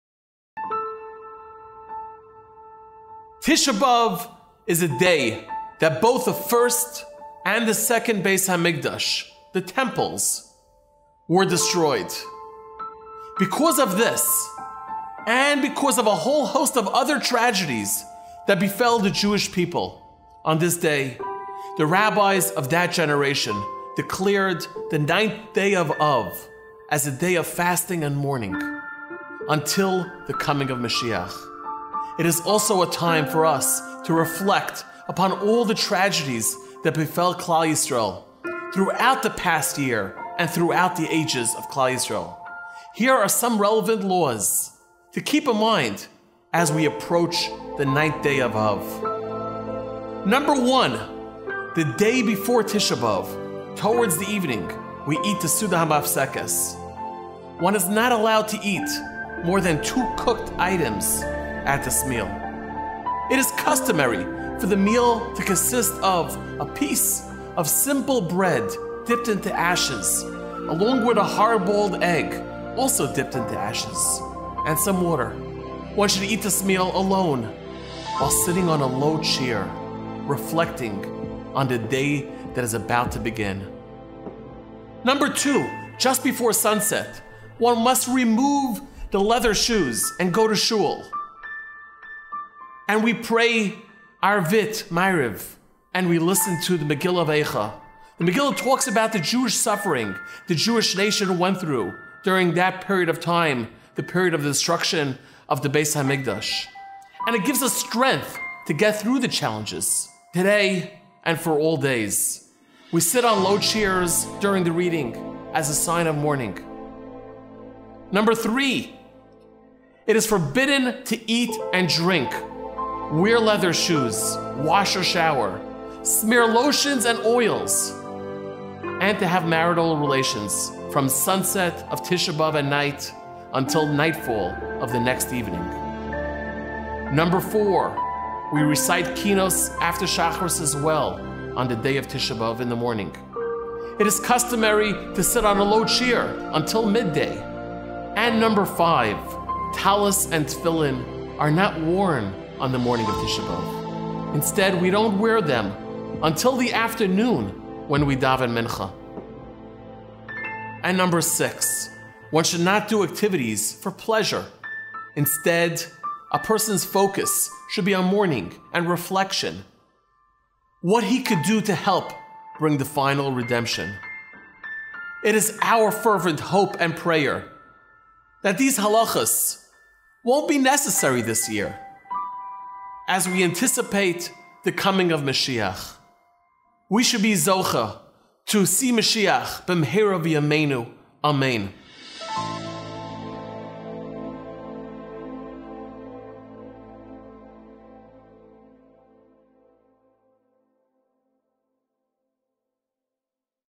Tisha B’av - An Overview by Rabbi Eli Bohm
What is it about the Ninth of Av that brings the Jewish nation to their knees? Hear from Rabbi Eli Bohm from TheZone about this day, its history, and its laws.